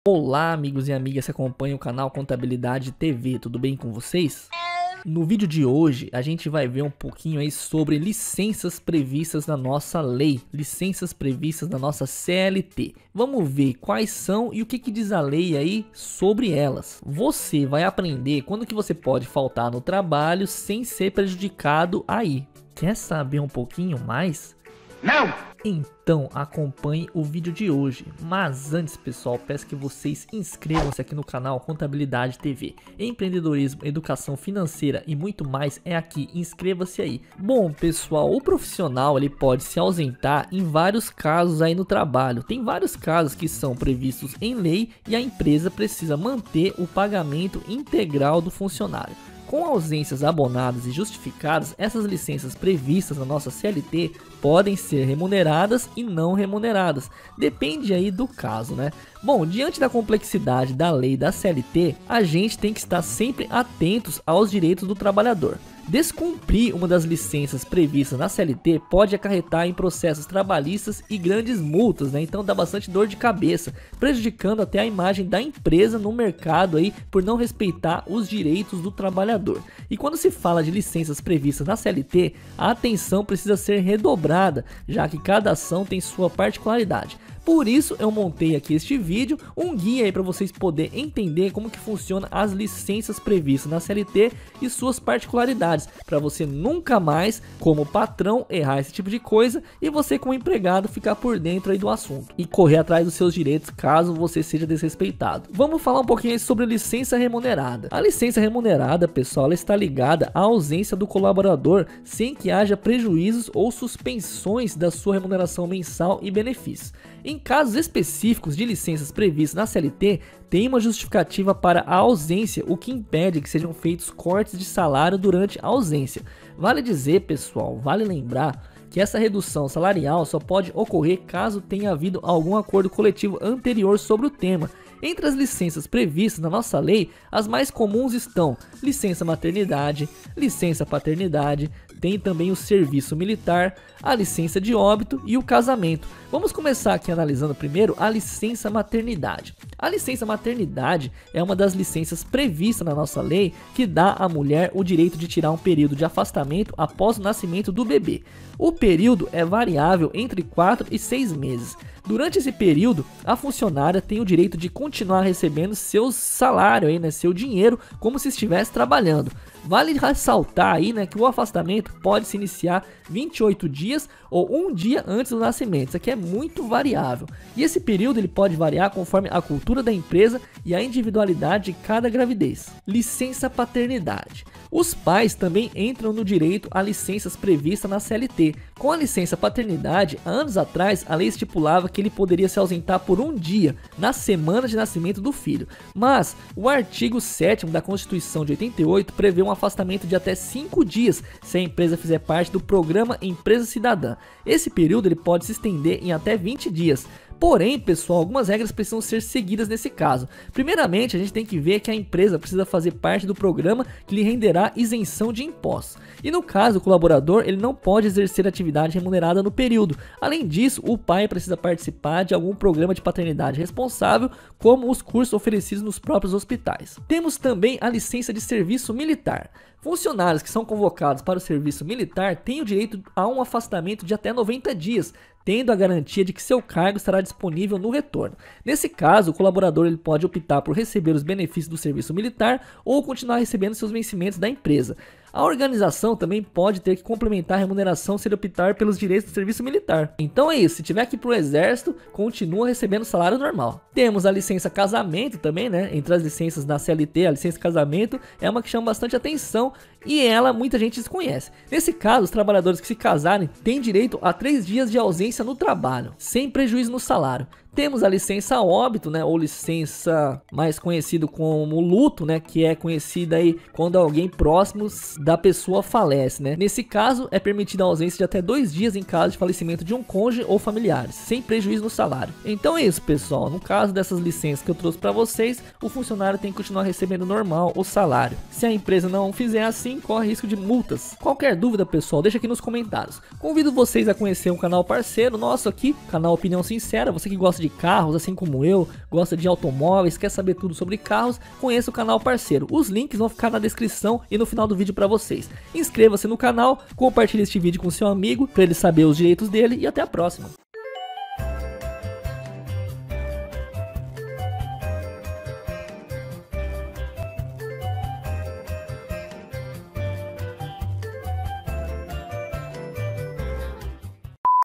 Quando posso faltar no trabalho sem ser prejudicado?
Conheças as Licenças previstas na CLT: Saiba quais são e o que diz a lei sobre faltar no trabalho e ainda ganhar dinheiro.

Saiba mais sobre: Licença maternidade, Licença paternidade
Serviço militar, Licença Casamento e Licença óbito